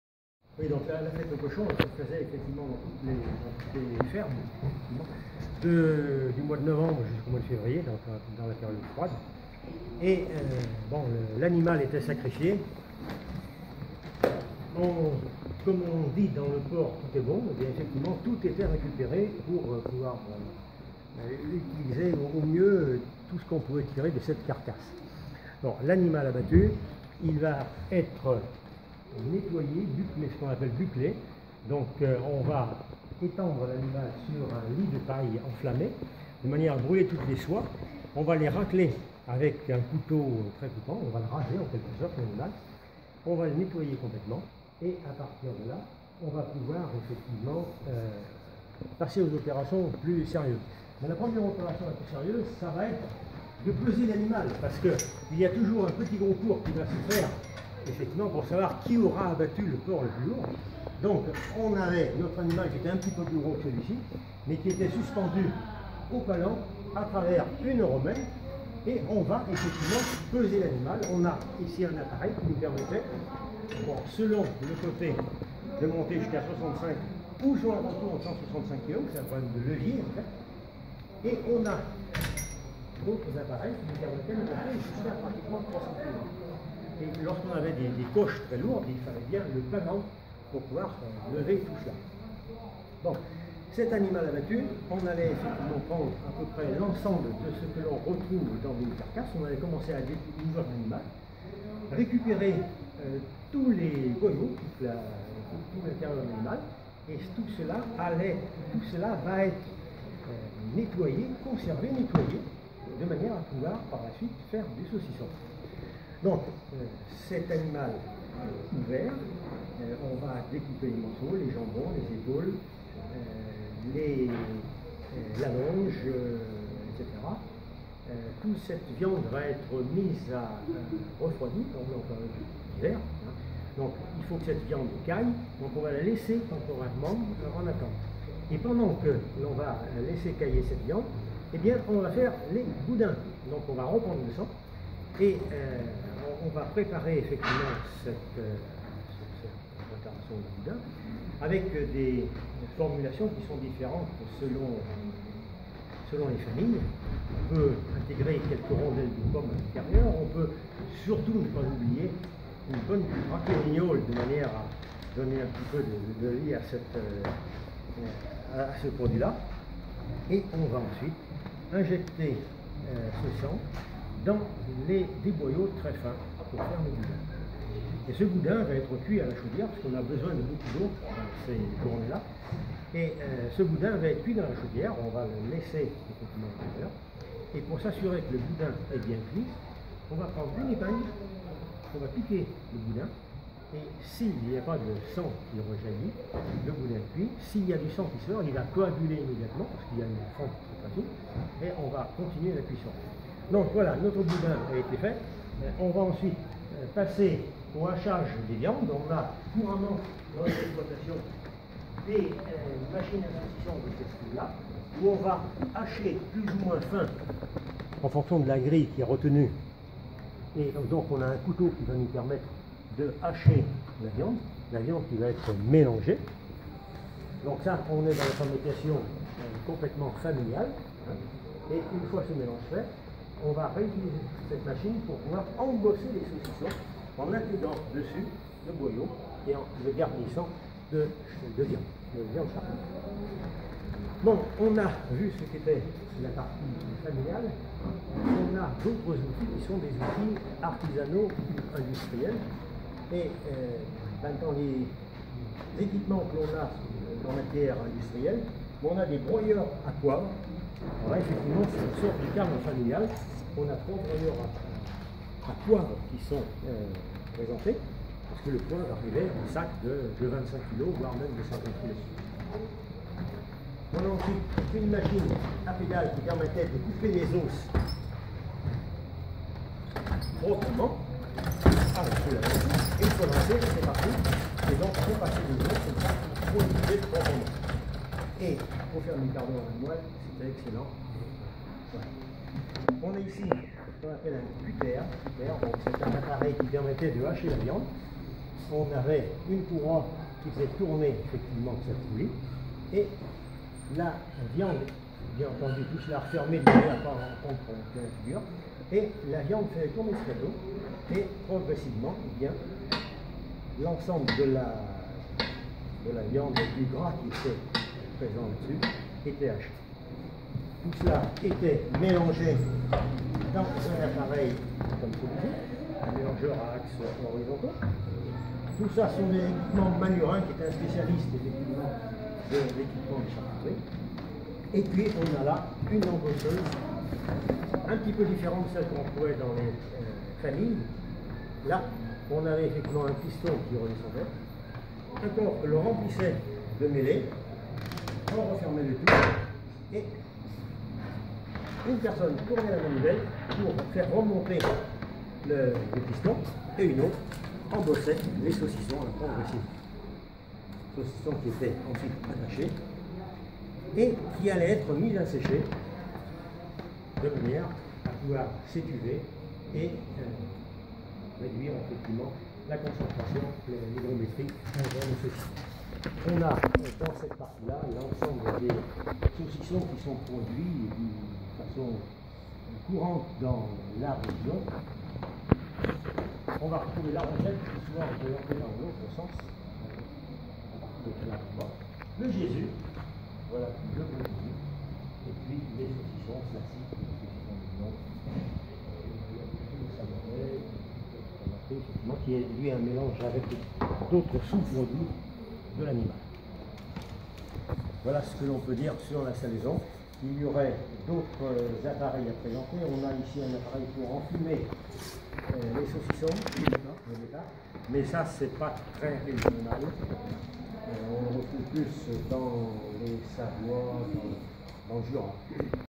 Présentation de l'atelier saucisson de la maison des métiers
Le dimanche, c'est patrimoine ! Vous avez toujours rêvé savoir comment était confectionné un saucisson de manière artisanale ?

Roger Peillon, ancien Maire et président de la confrérie de la Noble Rosette, du fin Saucisson et du bon Jésus vous explique tout cela en détail !

La démonstration a été faite ce jour pour un reportage sur le Progrès qui sera diffusé dans les prochaines semaines.